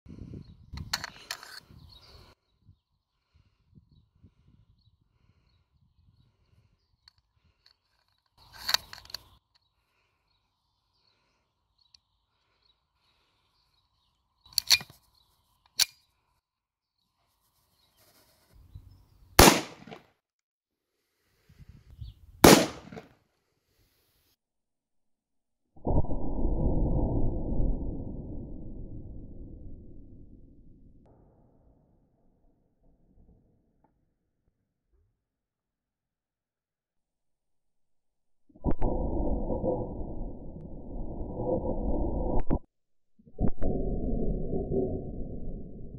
1911 A1 shooting Water (Slow Motion) #1911
In this video I am using the 'Colt Government 1911 A1' in 9mm P.A.K (Blank Gun)